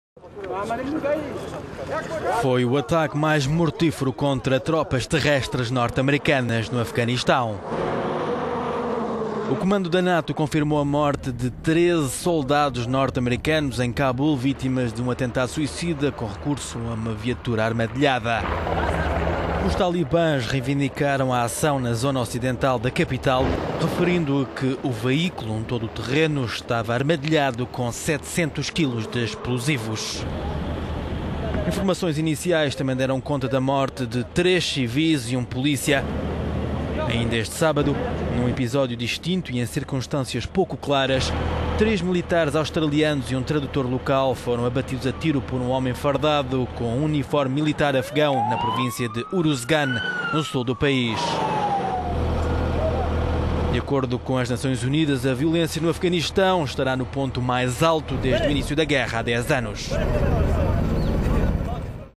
O pior ataque de sempre contra forças terrestres no Afeganistão
Foi o ataque mais mortífero contra tropas terrestres norte-americanas no Afeganistão. O comando da NATO confirmou a morte de 13 soldados norte-americanos em Cabul, vítimas de um atentado suicida com recurso a uma viatura armadilhada.

Os talibãs reivindicaram a ação na zona ocidental da capital referindo que o veículo, um todo-o-terreno, estava armadilhado com 700 quilos de explosivos.

Informações iniciais também deram conta da morte de três civis e um polícia.